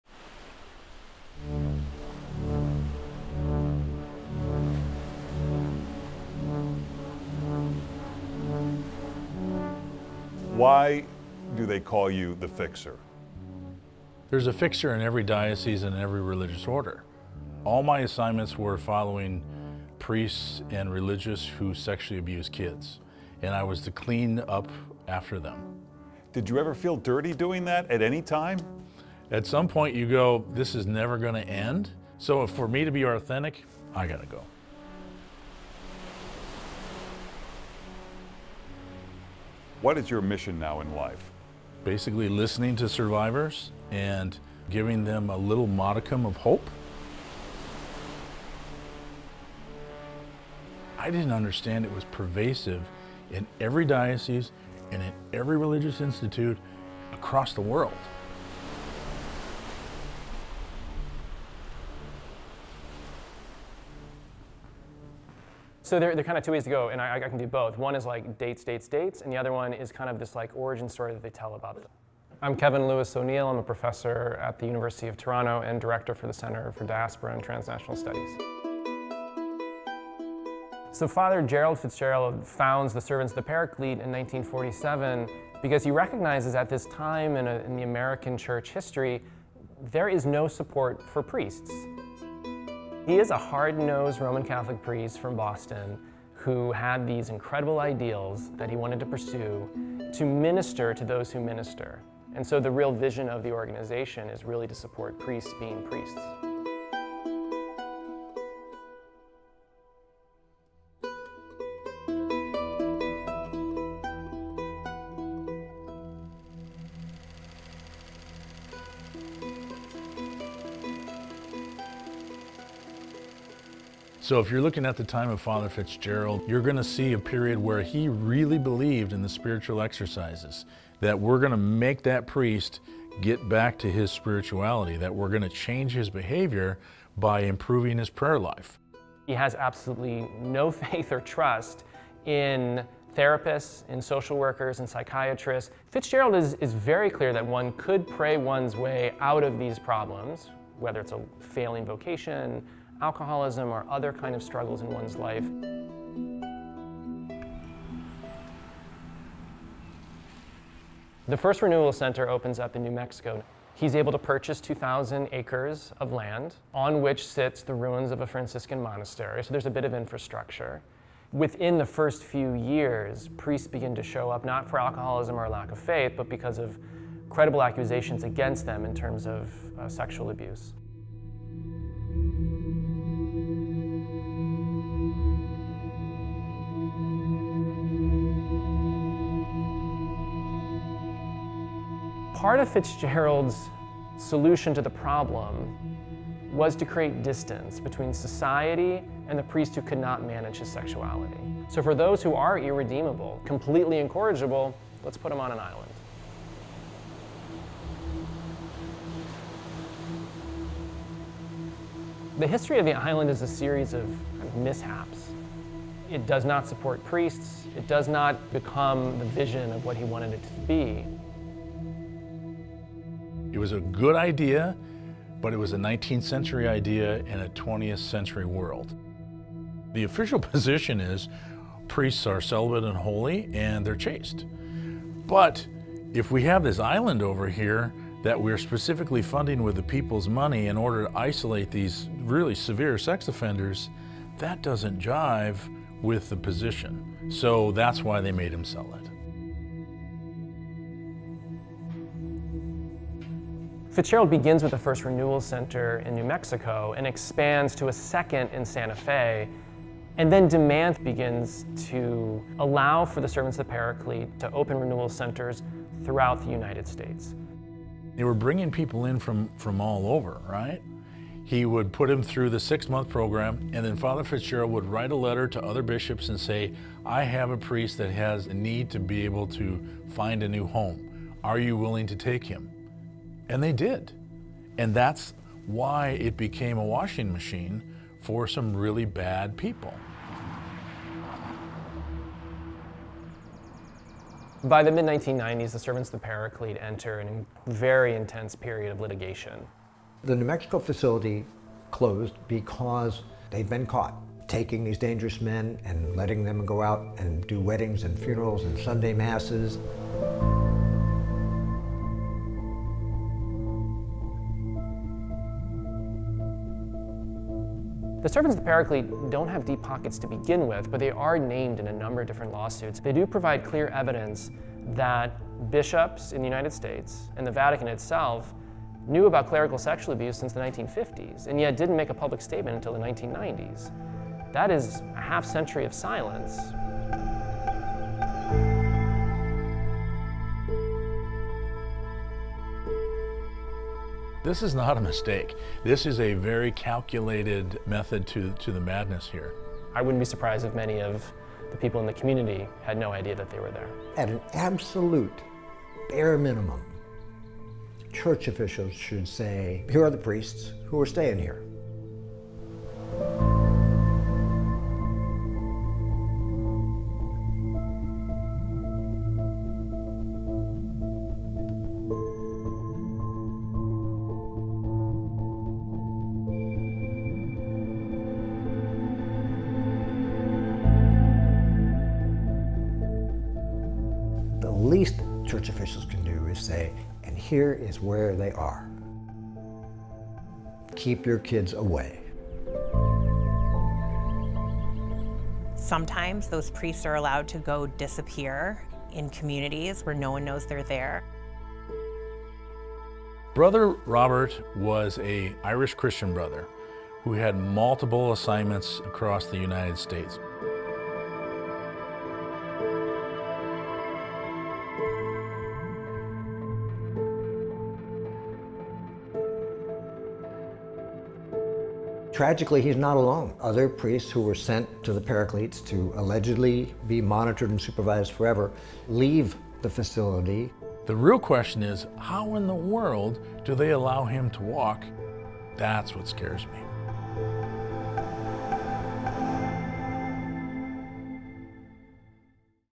Sanctuary of Sin Part 2: How a religious order became a sanctuary for pedophile priests
Father Gerald Fitzgerald started a Catholic religious order after a chance encounter with a beggar. It turned out the beggar was a priest in need of help and had nowhere to turn.

So, Father Fitzgerald founded the Servants of the Paraclete in the late 1940s to help fill that void. He established in a compound in the New Mexico desert where priests struggling with alcohol abuse or a crisis of faith could be treated.

But he soon encountered clergy with more serious issues